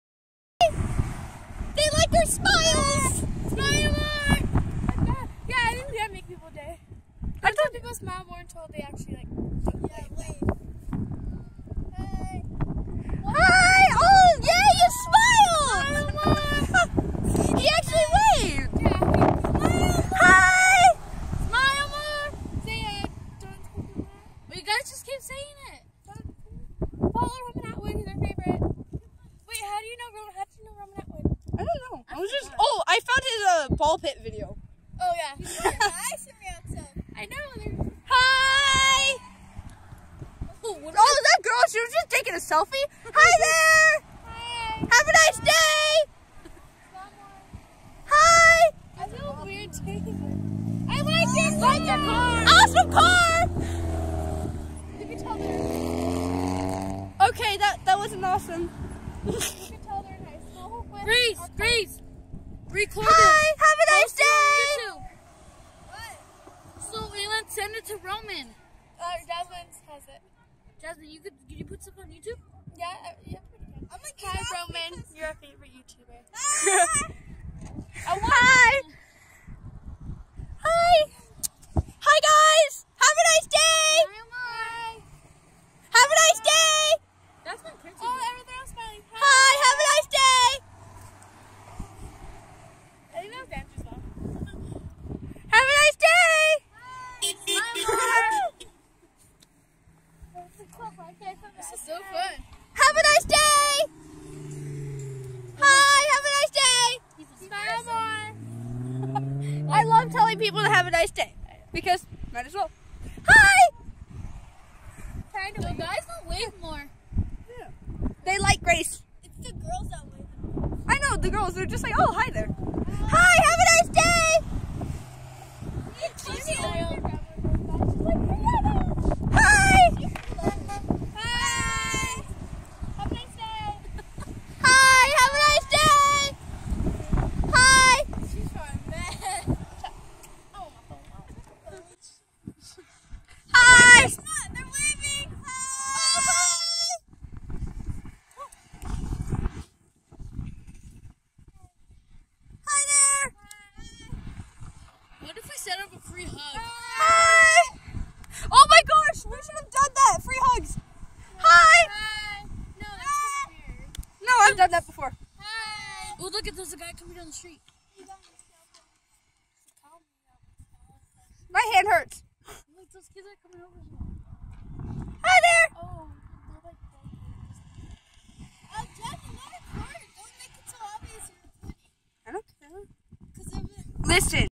Waving to passing cars
At a park, me my cousin and my brother and sister were waving at passing cars, telling them Happy Valentine's day and to have a good day. You can't see it, but when we were recording, people would wave back and smile. We kept saying smile more as a reference to Roman Atwood. We all love him here :)